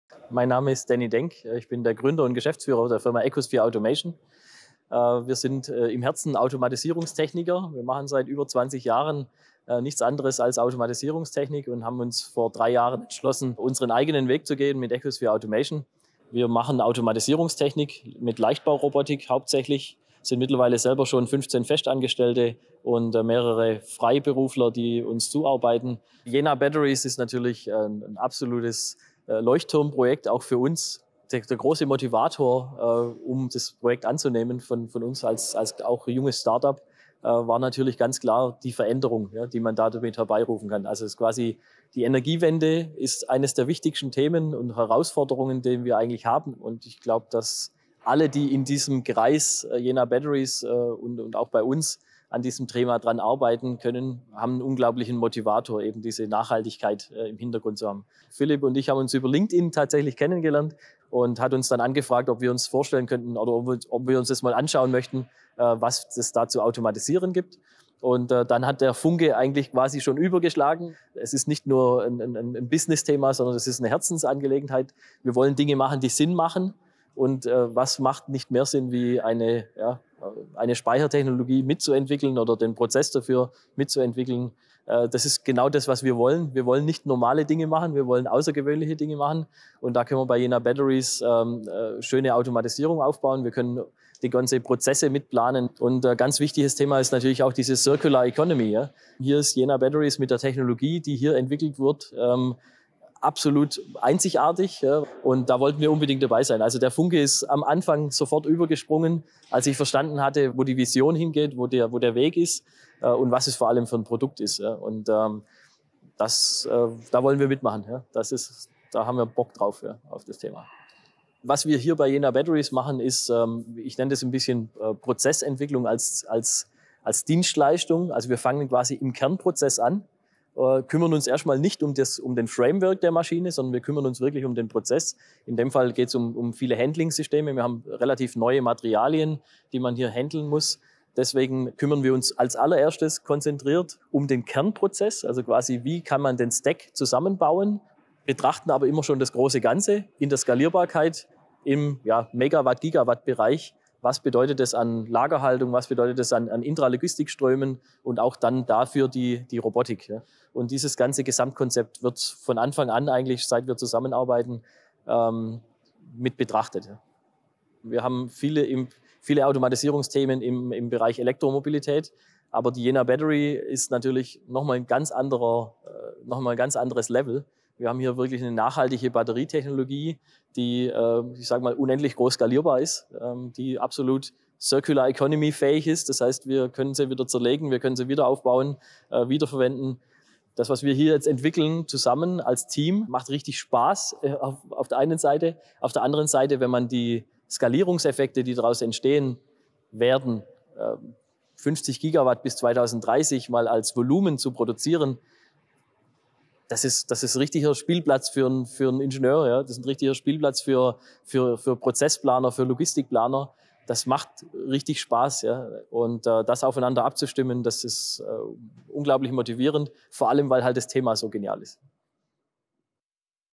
„Da haben wir Bock drauf!“ - Danny Denk (ECOSPHERE Automation) über unsere Zusammenarbeit
Wir blicken auf eine spannende Zusammenarbeit mit ECOSPHERE Automation, die uns tatkräftig bei dem Ausbau unserer Roboterlinie unterstützen. Wir haben Danny bei einem Besuch in unseren Produktionshallen dazu befragt, was die Arbeit so spannend macht.